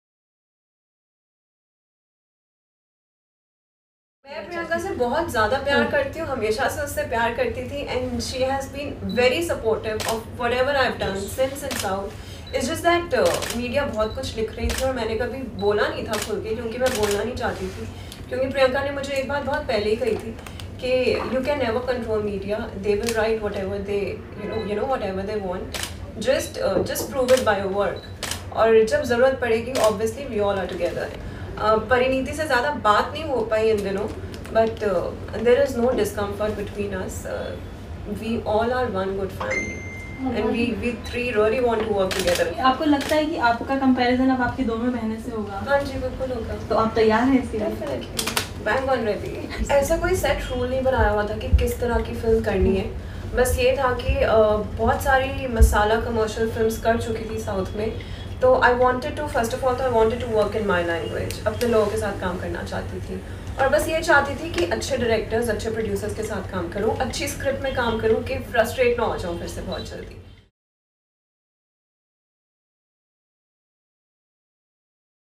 'No discomfort between Me, Priyanka & Parineeti' - Meera Chopra | Stars In The City
Meera Chopra speaks on being compared to her cousins, Priyanka and Parineeti Chopra, and her future projects.

Video by: Manas Gurung & Anish Das
Produced by: Manas Gurung & Anish Das